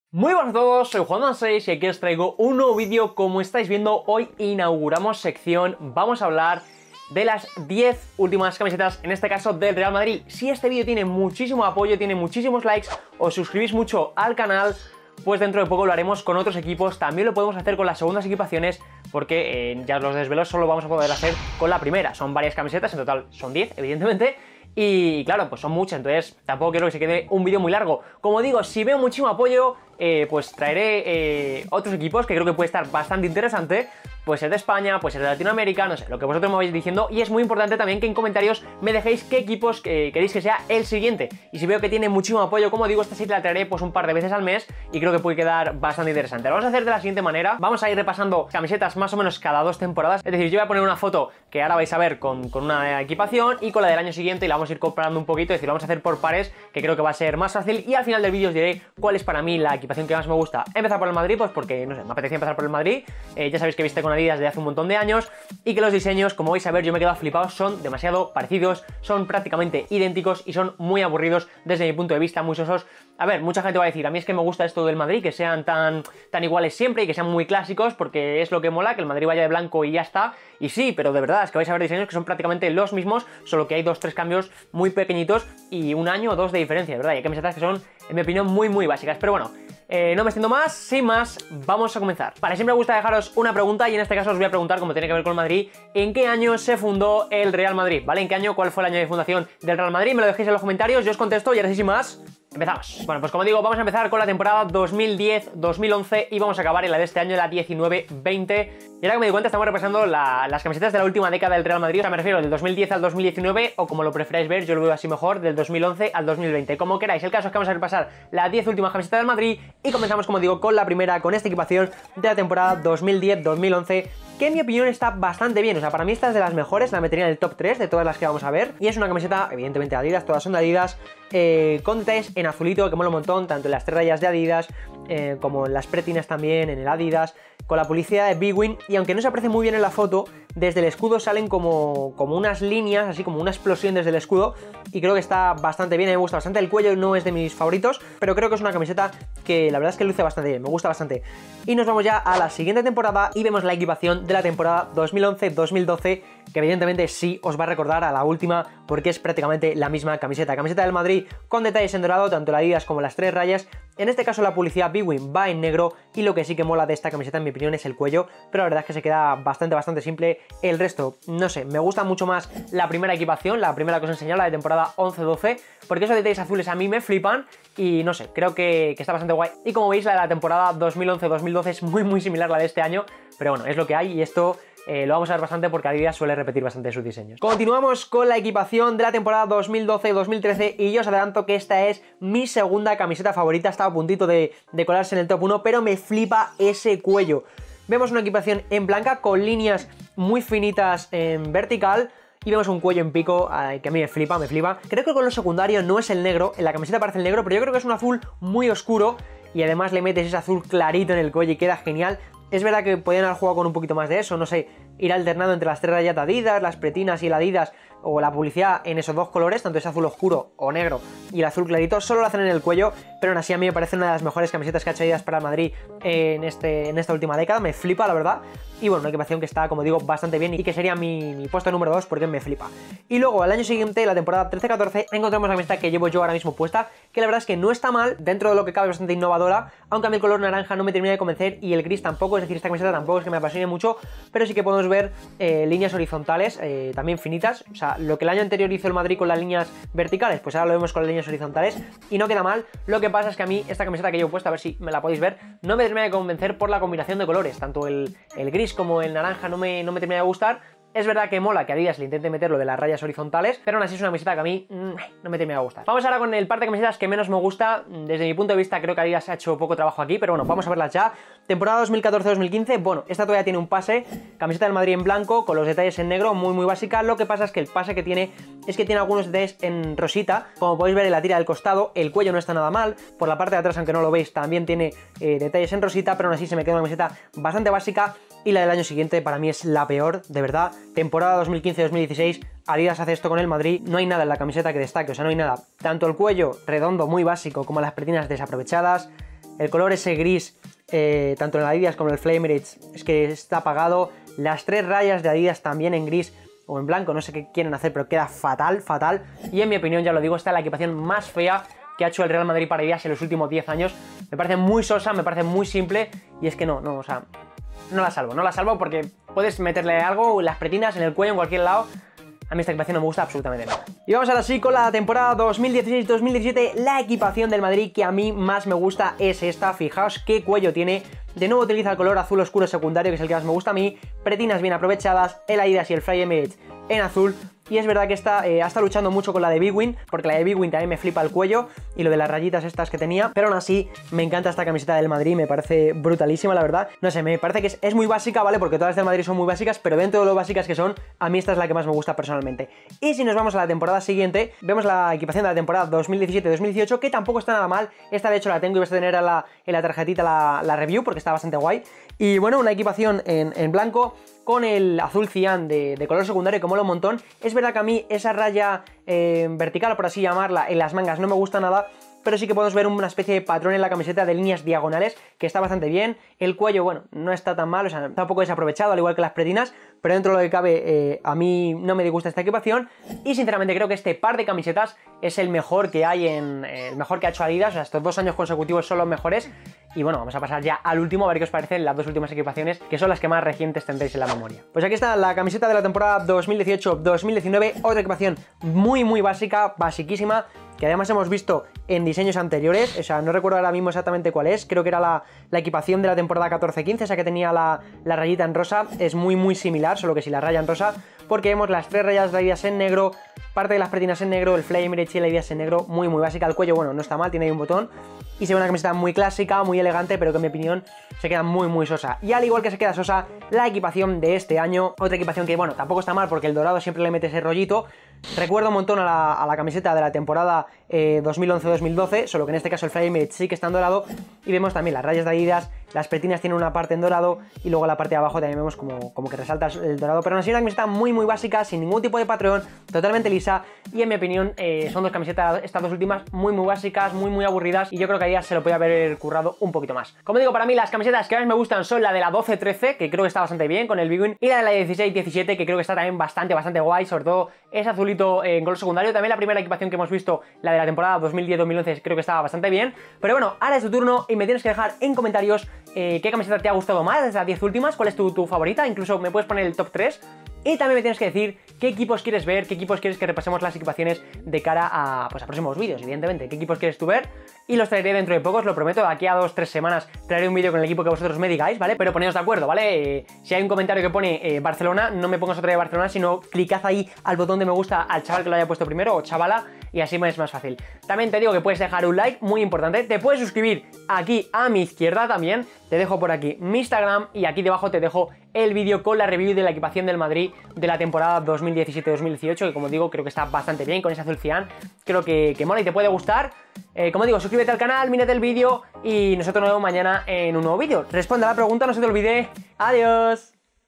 Las 10 ÚLTIMAS CAMISETAS del REAL MADRID CF con ADIDAS
Hoy echamos la vista atrás para ver LAS DIEZ ÚLTIMAS CAMISETAS del REAL MADRID con ADIDAS